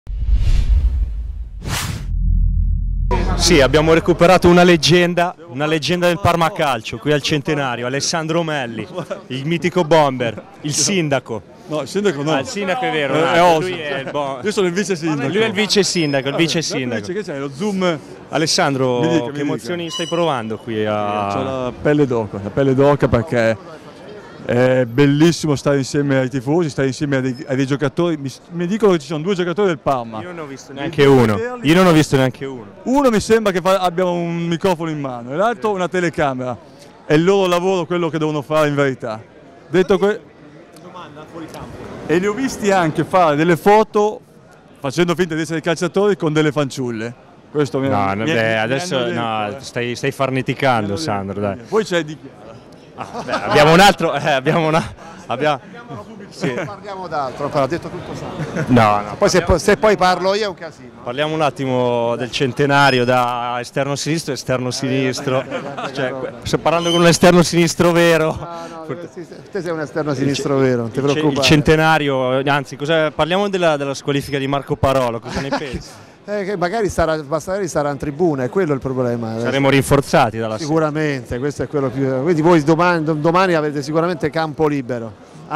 Parolo in camera, Gobbi al microfono: le loro interviste speciali a Melli-Di Chiara
Marco Parolo in camera, Massimo Gobbi con il microfono. Ancora loro due, scatenati su Parma Channel. Stavolta tocca a Melli e Di Chiara rispondere alle loro domande...